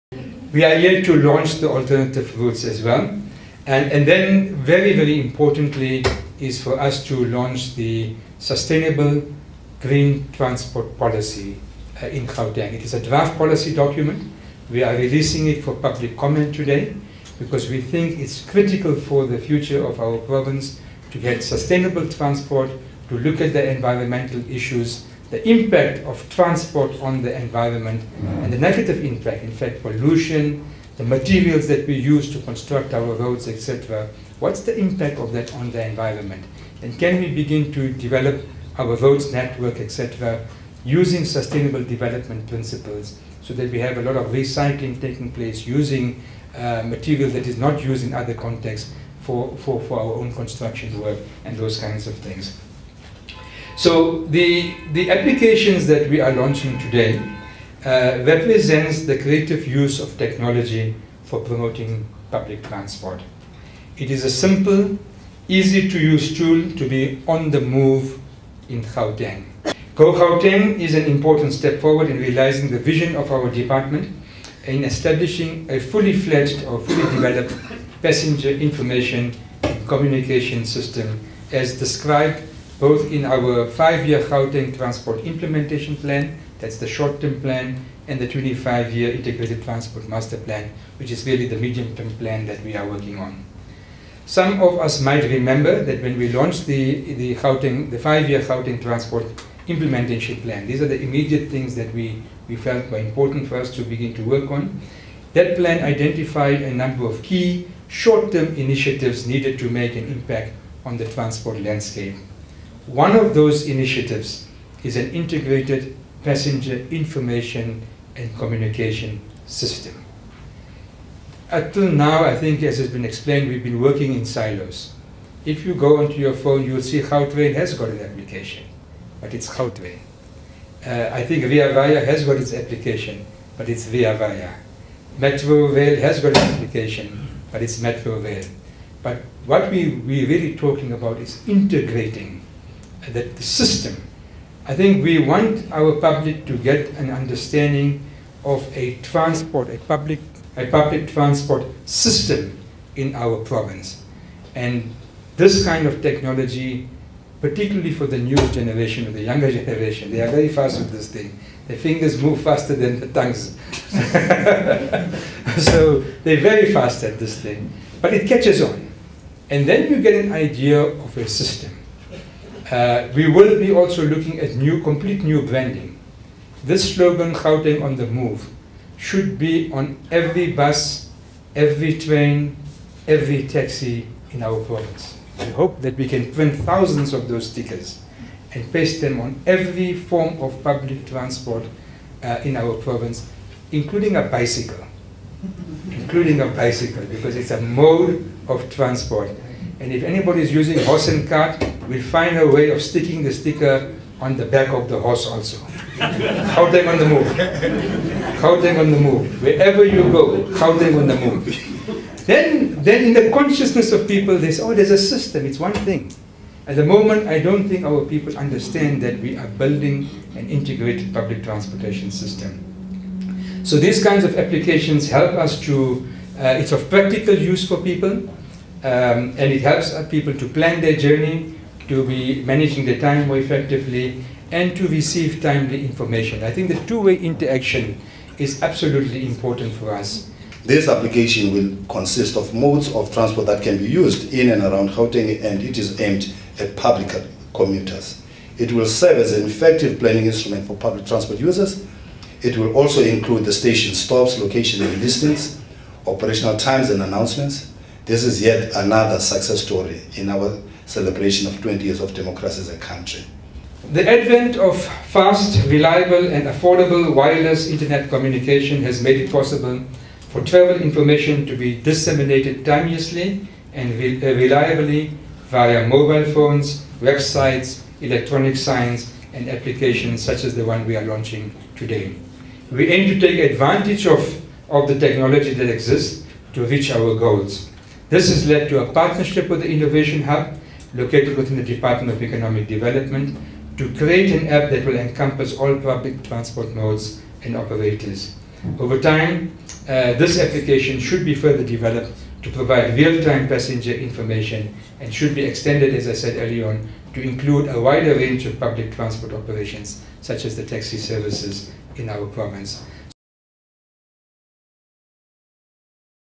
GPG launches GoGauteng app, green transport draft policy
The Gauteng provincial government (GPG) on Wednesday launched the GoGauteng mobile application (app) aimed at assisting commuters in planning their journeys, an alternative route app to provide road users with the resources needed to avoid e-toll roads and also released the 'Promoting Sustainable (Green) Transport in Gauteng' draft policy for public comment.